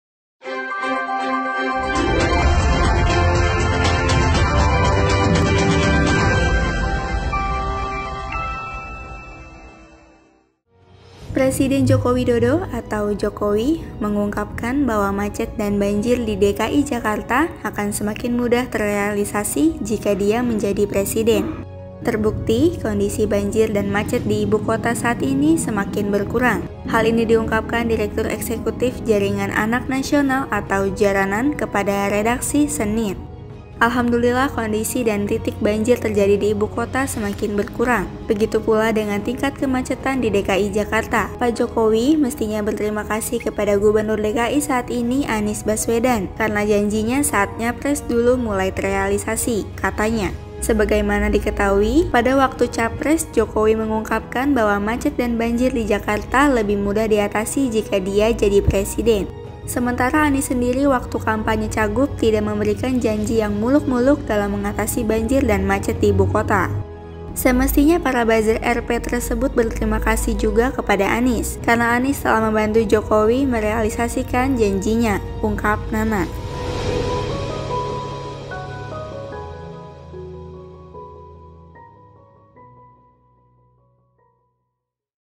ANIES WUJUDKAN MIMPI JOKOWI
Presiden Joko Widodo (Jokowi) mengungkapkan bahwa macet dan banjir di DKI Jakarta akan semakin mudah terealisasi jika dia menjadi presiden. Terbukti, kondisi banjir dan macet di ibu kota saat ini semakin berkurang. Hal ini diungkapkan  Direktur Eksekutif Jaringan Anak Nasional (Jaranan) kepada redaksi, Senin (22/2/21).

“Alhamdulillah, kondisi dan titik banjir terjadi di Ibu Kota semakin berkurang. Begitu pula dengan tingkat kemacetan di DKI Jakarta. Pak Jokowi mestinya berterima kasih kepada Gubernur DKI saat ini (Anies Baswedan), karena janjinya saat nyapres dulu mulai terealisasi,” katanya.

Sebagaimana diketahui, lanjut penggagas Klub Literasi Progresif (KLip) ini, pada waktu capres, Jokowi mengungkapkan bahwa macet dan banjir di Jakarta lebih mudah diatasi jika dia jadi Presiden. Sementara Anies sendiri waktu kampanye cagub, tidak memberikan janji yang muluk-muluk dalam mengatasi banjir dan macet di ibu kota.

“Semestinya para BuzzerRp tersebut berterima kasih juga kepada Anies. Karena Anies telah membantu Jokowi merealisasikan janjinya,” ungkap Nanang.